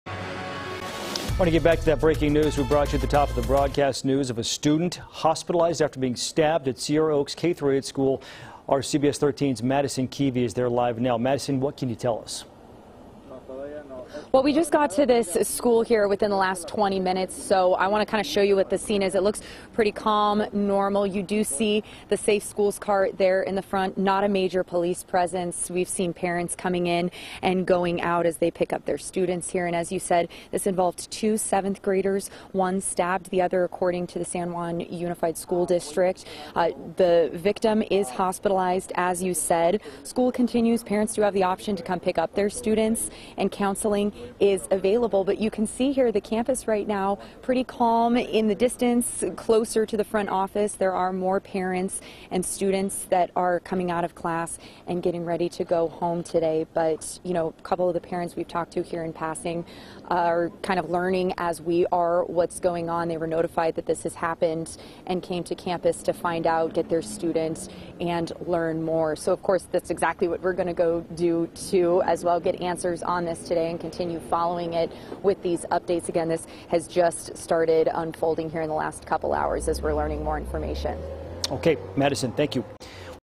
Student Accused Of Stabbing Other Student At Sacramento School
One student has been stabbed at Sierra Oaks K-8 school in Sacramento on Thursday.